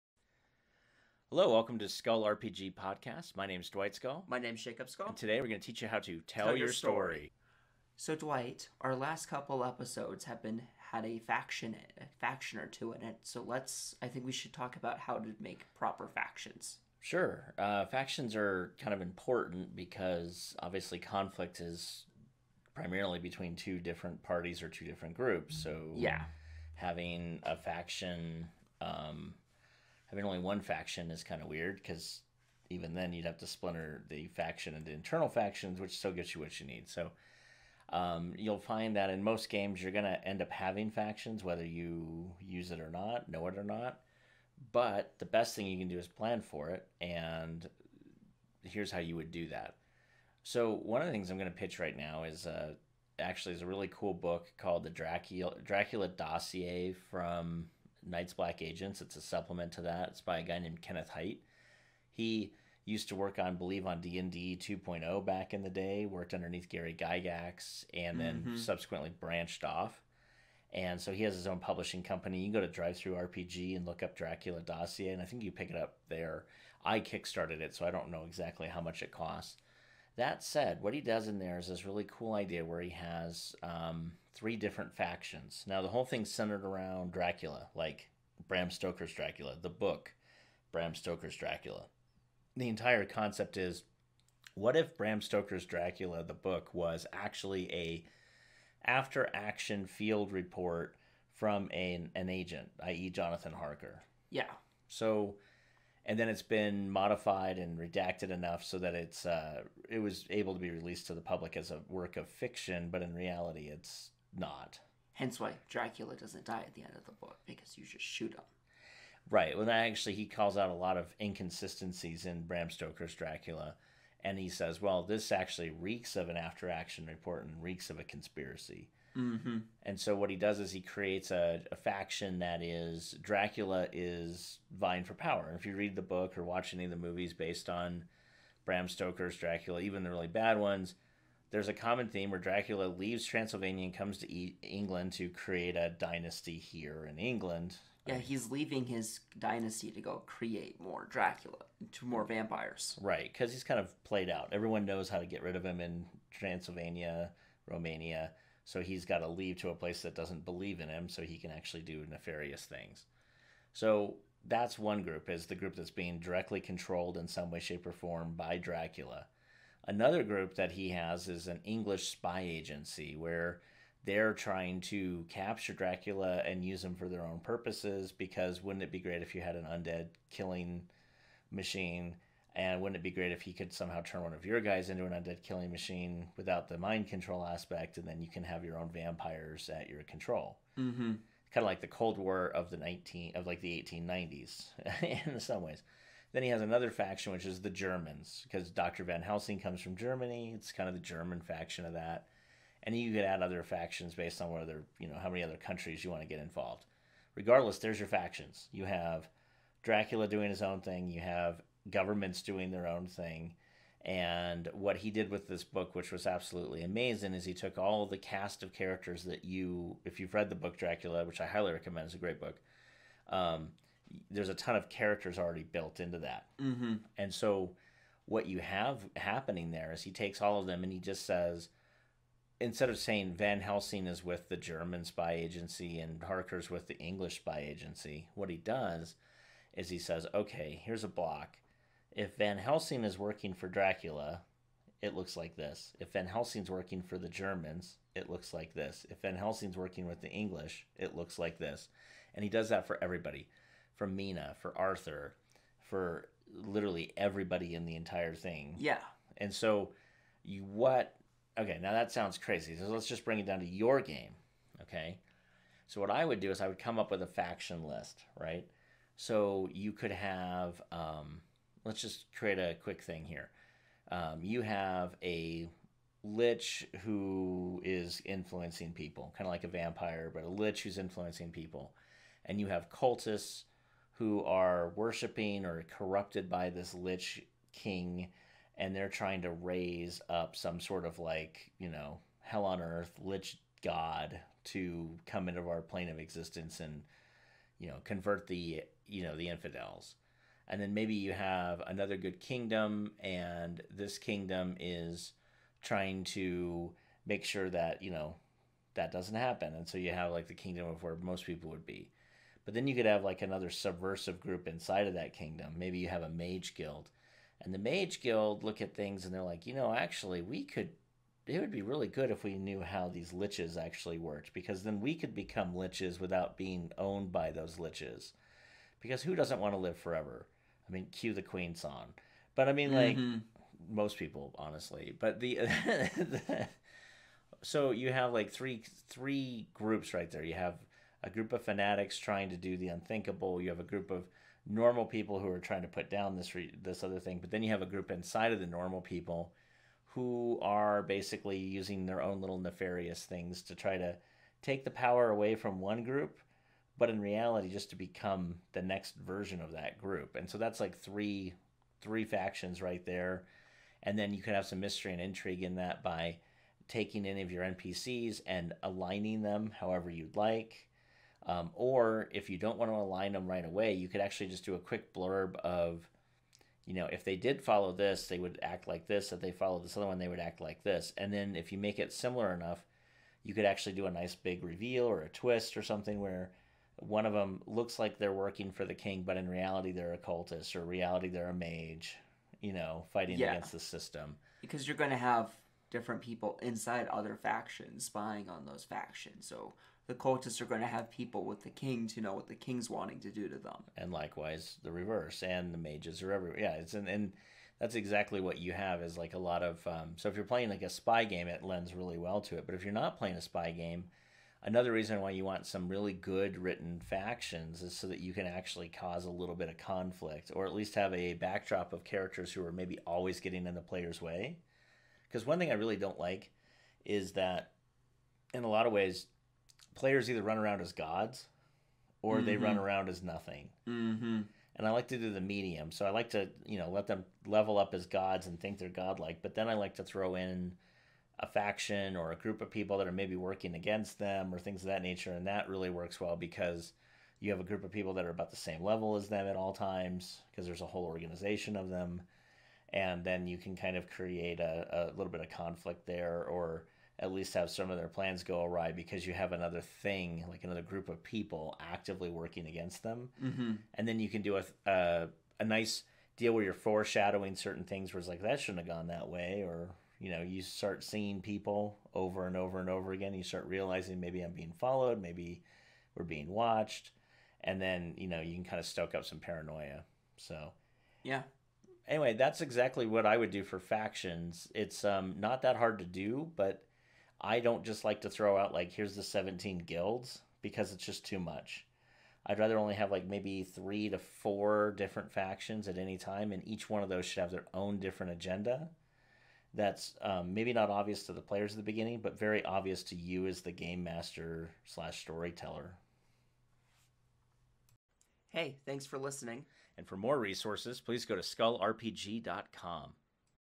How to create 3 or more different factions in your RPG
Hey Storytellers,

There are times when you want to explore different guilds or factions in your RPG. Every good conflict needs at least two parties, but by developing 3 or 4 different factions in your world.

Giving flesh to your different factions allows you to build a world that is more real. One side is working against another. Players potentially hired by one or more for jobs or spying. By having 3 or more factions or guilds that have motives and goals of their own you will be able to create small or large conflicts and move your story along. You will also have the ability to do doublecrosses and backstabbing with these groups and your players.